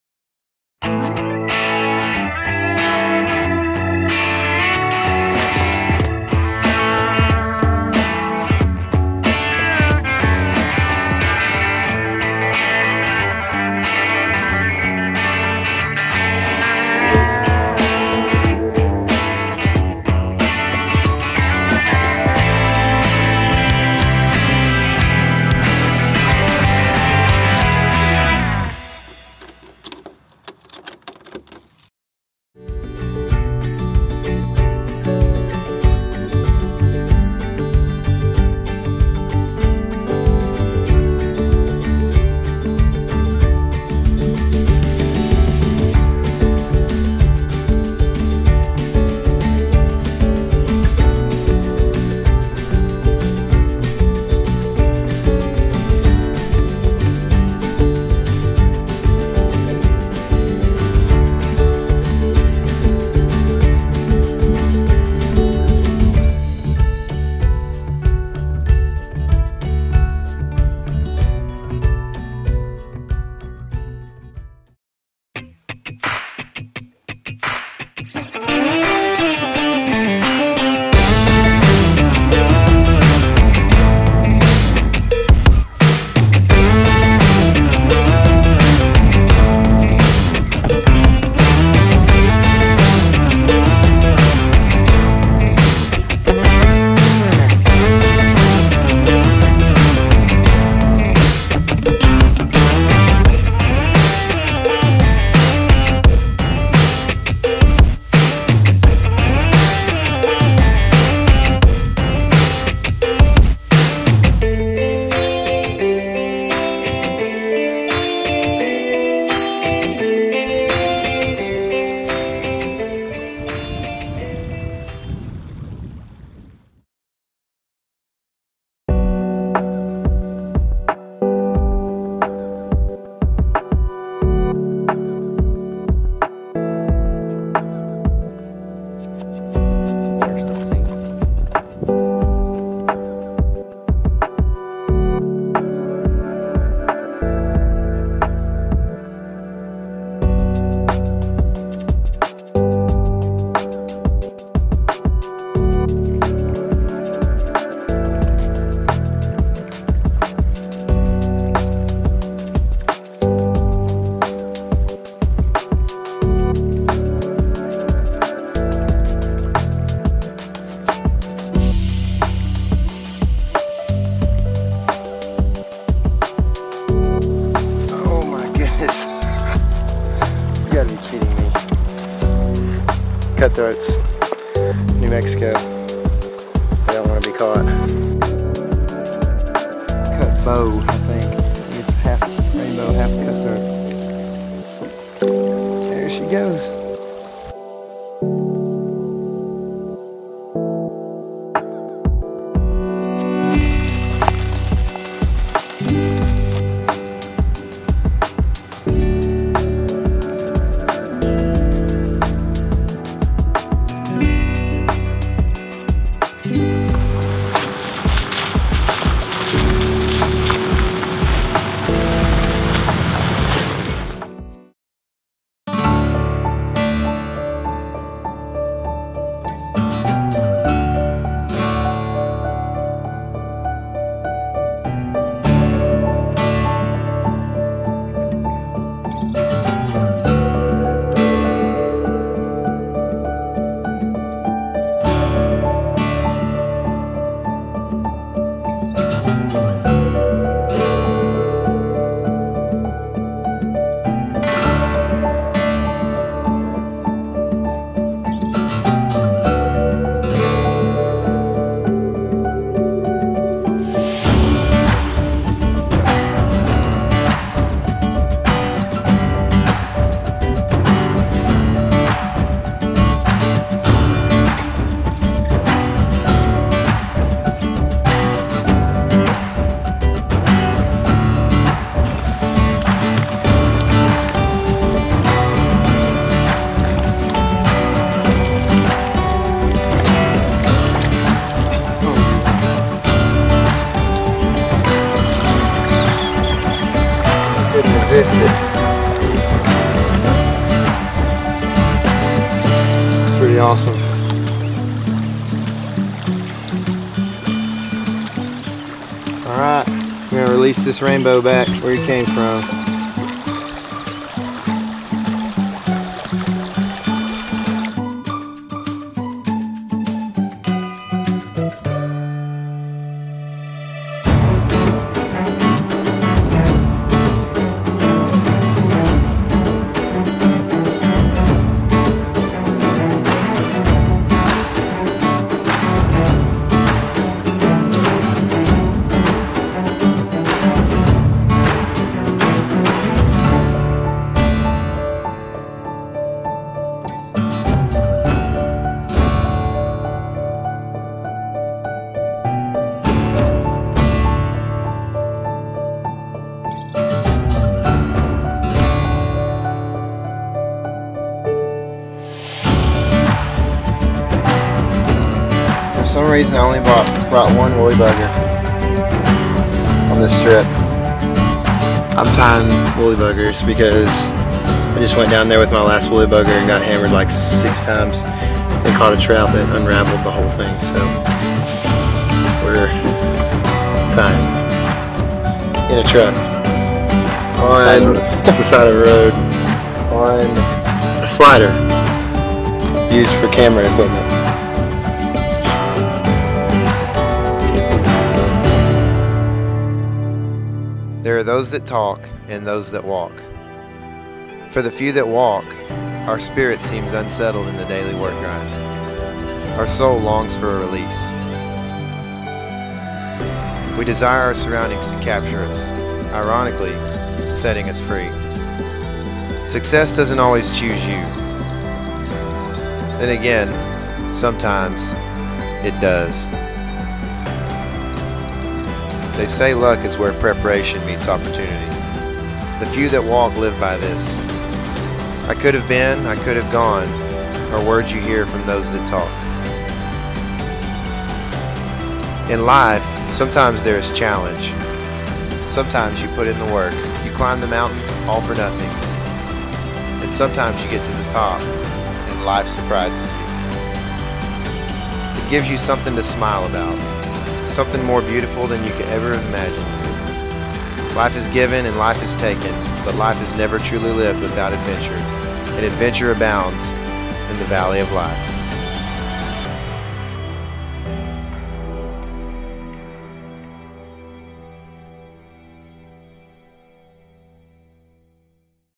Valley of Life
"Life is given and life is taken, but life is never truly lived without adventure and adventure abounds in the Valley of Life."

Over 30 miles from pavement in a remote section of Northern New Mexico's Valle Vidal, huge elk herds can be found feeding around the fertile stretches of creek where wild and native Rio Grande Cutthroat trout hold on to a small percentage of their original range. A few days here will make you never want to go back.